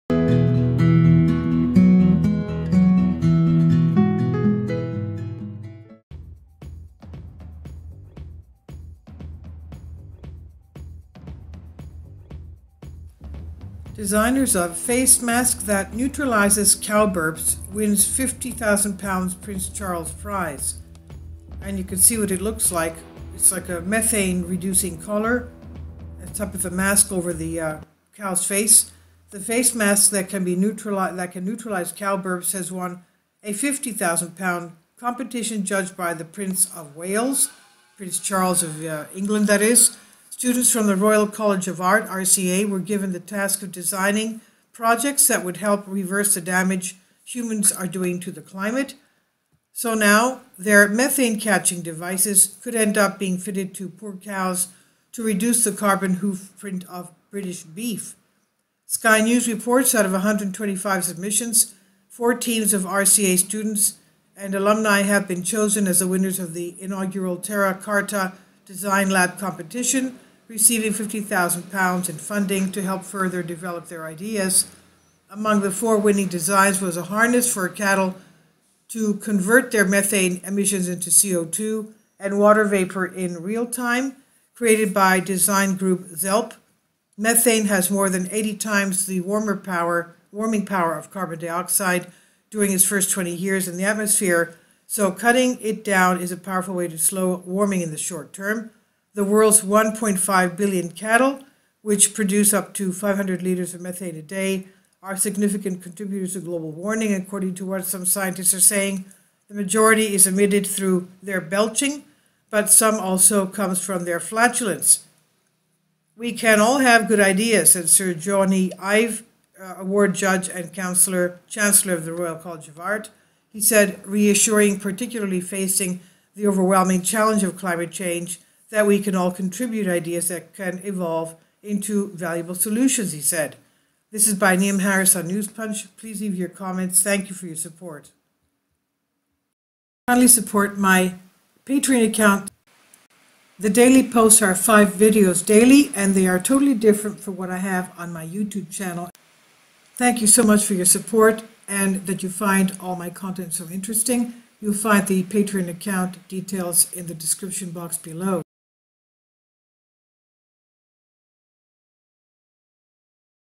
Designers Of Face Mask That Neutralises Cow Burps METHANE GAS Win £50k Prince Charles Prize!
Please Support:
Thumbnail image - cow Methane-Reducer-Wearbable-5.jpg ZELP